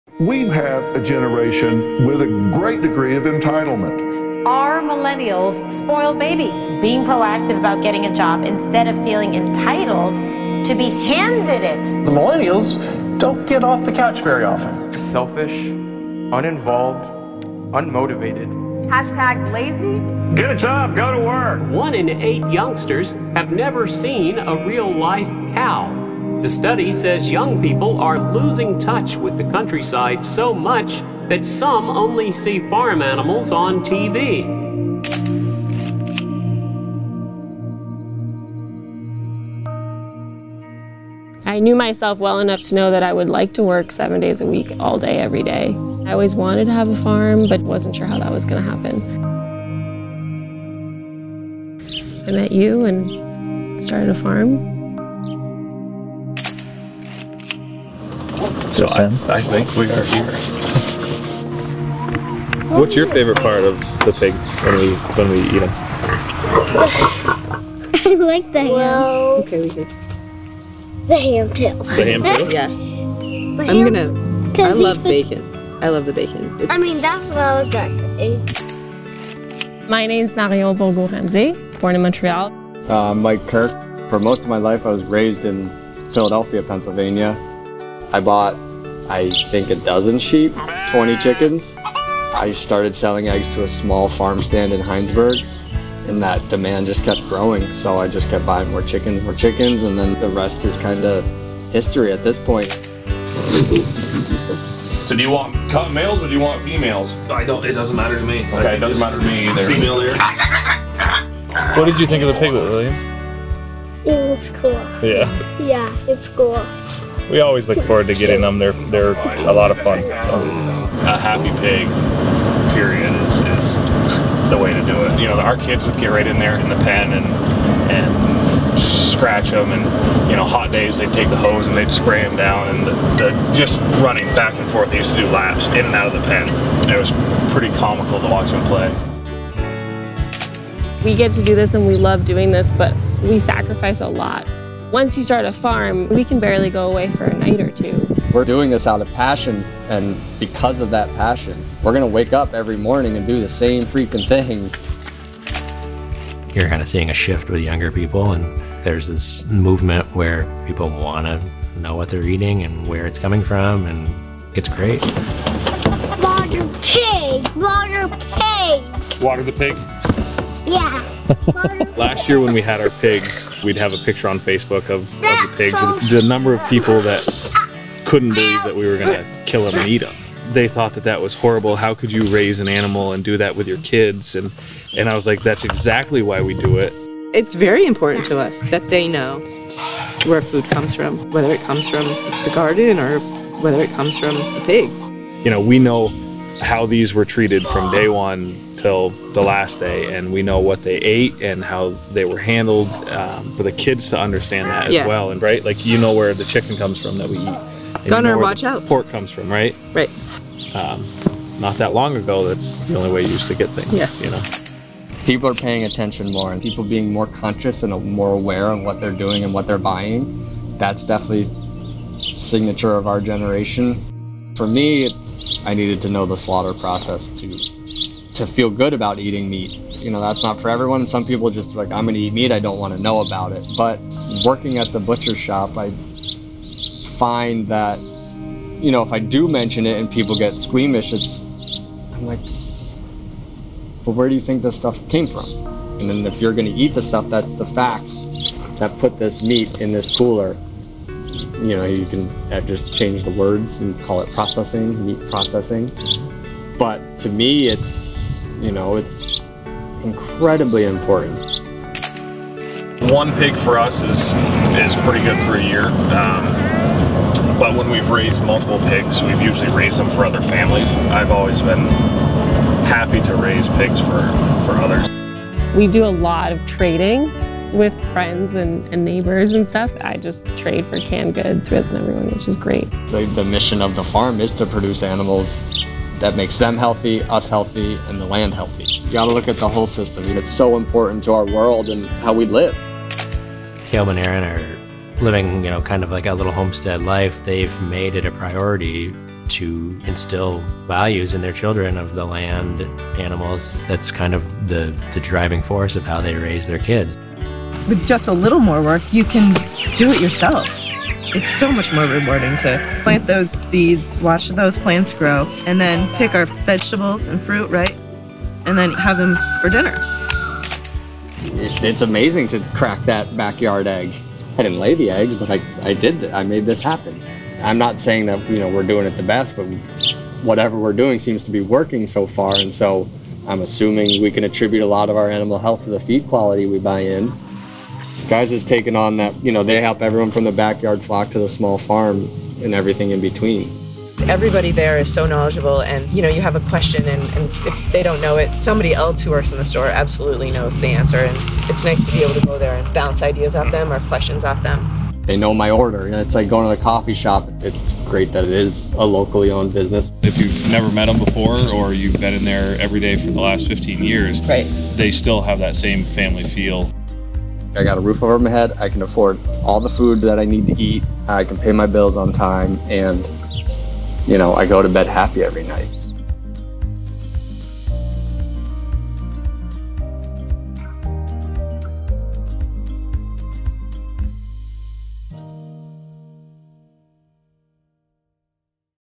The Millennial Movement
Highlighting two local Families and how millennials are impacting and maintaining Vermont’s farming culture.